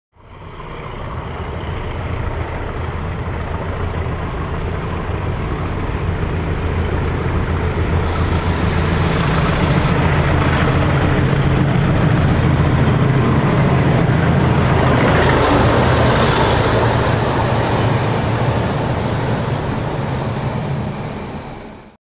GBRF CLASS 20s IN NEW LIVERY
This was the first run with the class 20s in there new livery working 7X09 to Amersham.